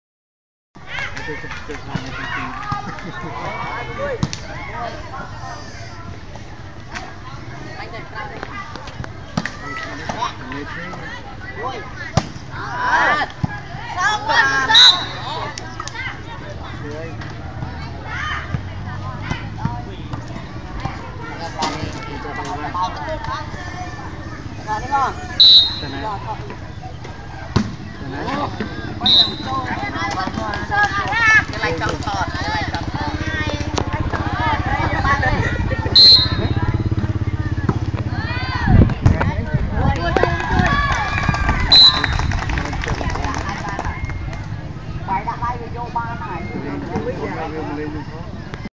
MOK NEAK schoolសៀមរាប Vs កពង់ស្ពឺ ឆ្នាំ2015
សៀមរាប Vs កំពង់ស្ពឺ នៅខេត្ត ស្វាយរៀង ដឹកនាំដោយលោកគ្រូ ជា សំណាង នៃសាលាមុខនាគ